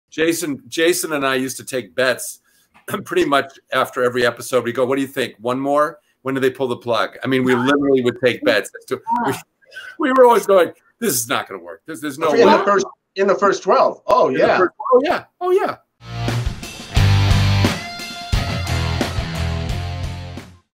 James Eckhouse and Jason Priestley Place Their Bets!
In Season 1, the two actors would bet on when the show would end!